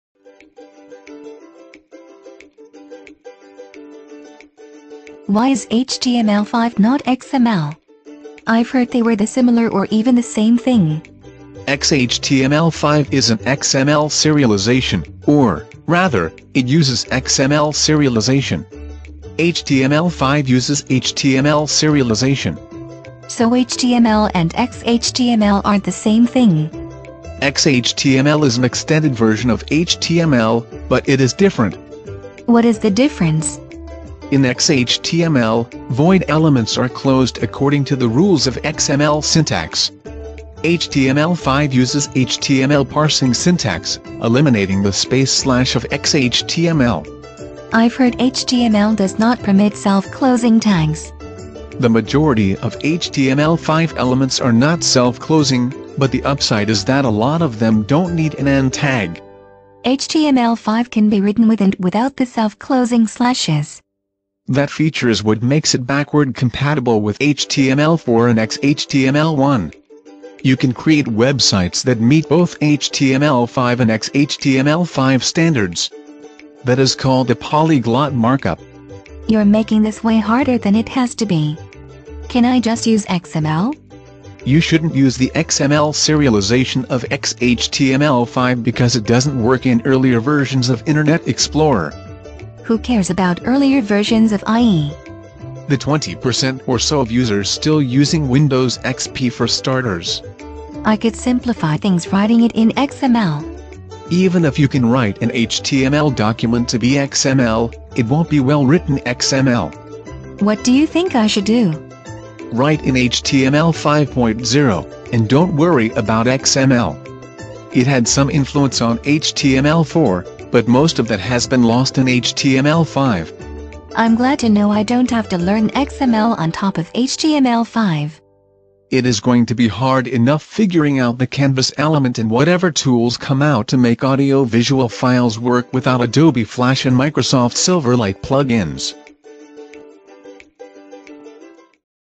Why is HTML5 not XML?
Why is HTML5 not XML? I've heard they were the similar or even the same thing.

XHTML5 is an XML serialization, or, rather, it uses XML serialization. HTML5 uses HTML serialization.

So HTML and XHTML aren't the same thing.

XHTML is an extended version of HTML, but it is different.

In XHTML, void elements are closed according to the rules of XML syntax. HTML5 uses HTML parsing syntax, eliminating the space slash of XHTML.

I've heard HTML does not permit self-closing tags.

The majority of HTML5 elements are not self closing, but the upside is that a lot of them don't need an end tag.

HTML5 can be written with and without the self-closing slashes.

That feature is what makes it backward compatible with HTML4 and XHTML 1. You can create websites that meet both HTML5 and XHTML5 standards; that is called a polyglot markup.

You're making this way harder than it has to be. Can I just use XML?

You shouldn't use the XML serialization of XHTML5 because it doesn't work in earlier versions of Internet Explorer.

Who cares about earlier versions of IE?

The 20% or so of users still using Windows XP for starters.

I could simplify things writing it in XML.

Even if you can write an HTML document to be XML, it won't be well written XML.

What do you think I should do?

Write in HTML 5.0, and don't worry about XML. It had some influence on HTML4, but most of that has been lost in HTML5.

I'm glad to know I don't have to learn XML on top of HTML5.

It is going to be hard enough figuring out the Canvas element and whatever tools come out to make audio-visual files work without Adobe Flash and Microsoft Silverlight plug-ins.